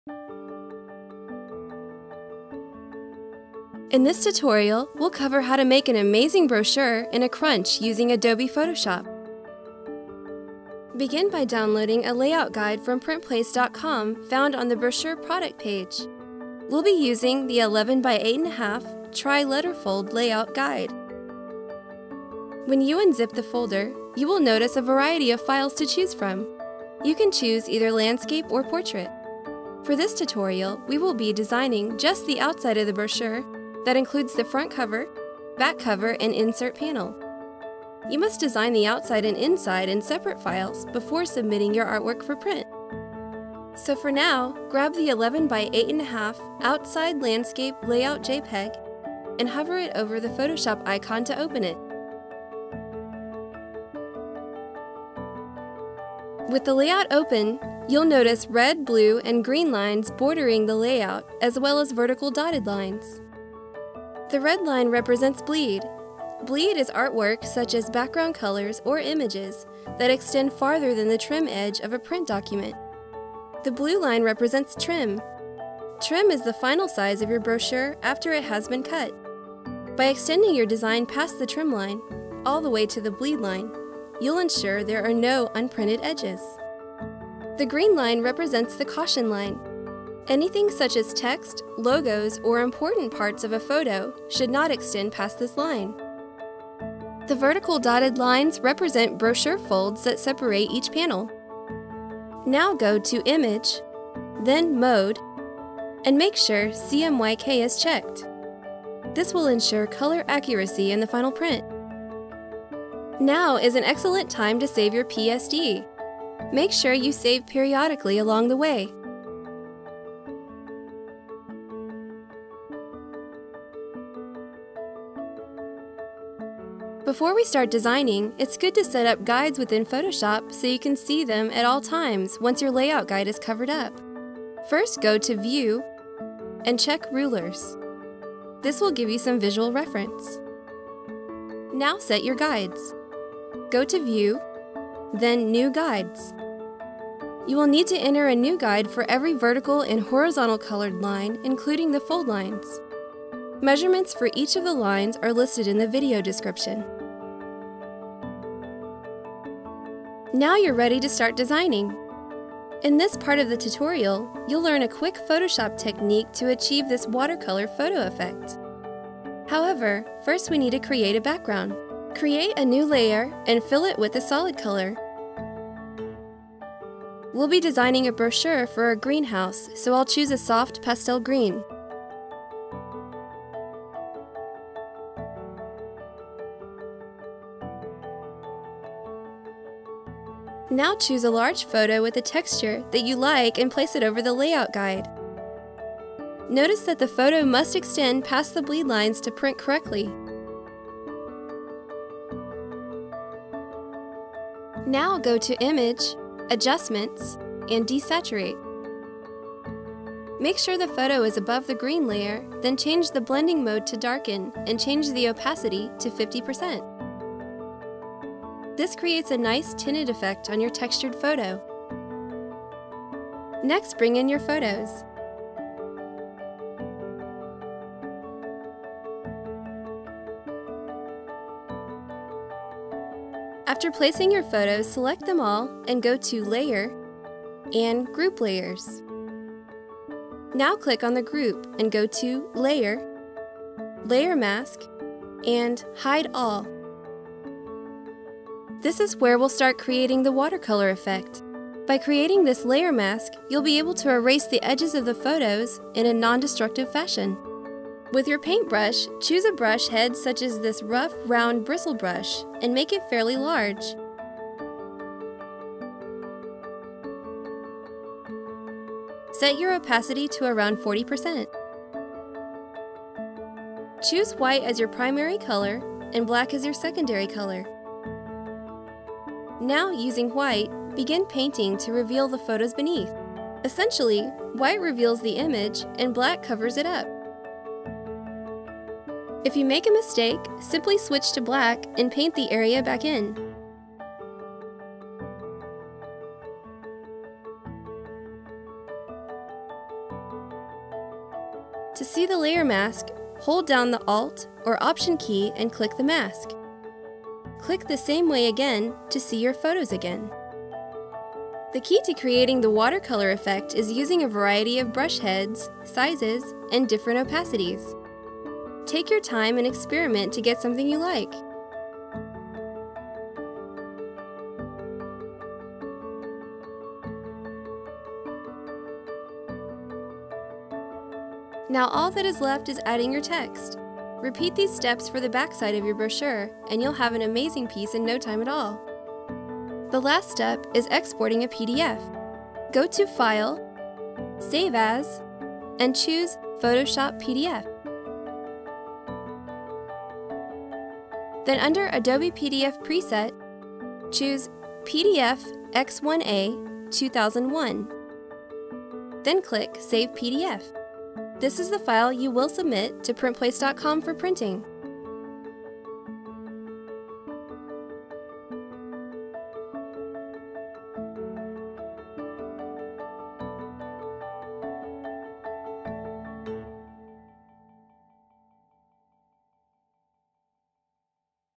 Create an Amazing Brochure in Photoshop | Graphic Design Tutorials from PrintPlace.com
- Learn how to create a quick amazing brochure in Adobe Photoshop. Learn how to make a water color photo effect and text that looks like water.

Measurements for Guides
Vertical: 375, .5, .625, 4.125, 7.8125, 11.375, 11.5, and 11.625
Horizontal: .375, .5, .625, 8.875, 9, and 9.125